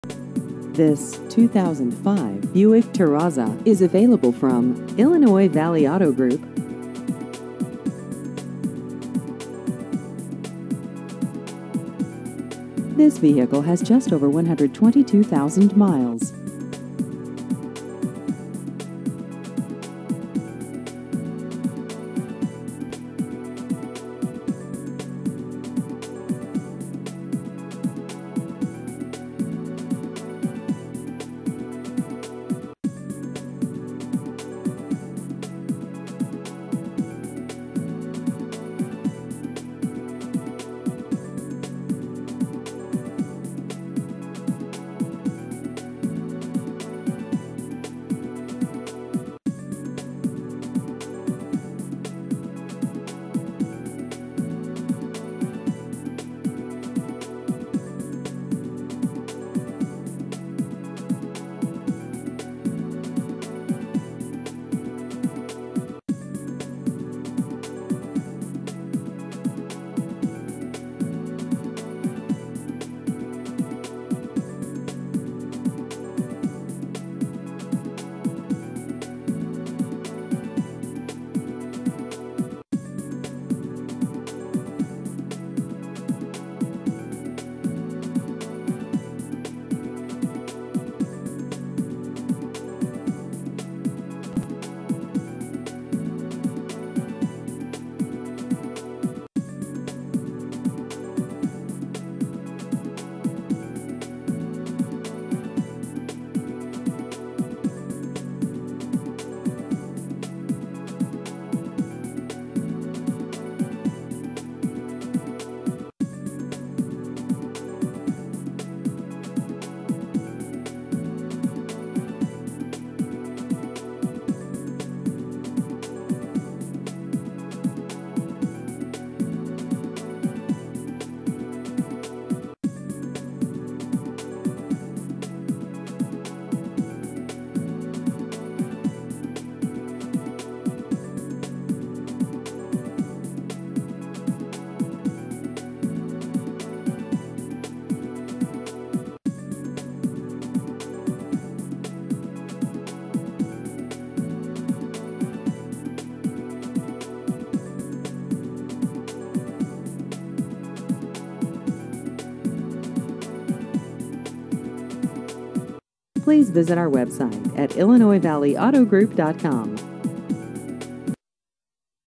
2005 Buick Terraza Used Cars Illinois Valley IL
This 2005 Buick Terraza is available from Illinois Valley Auto Group.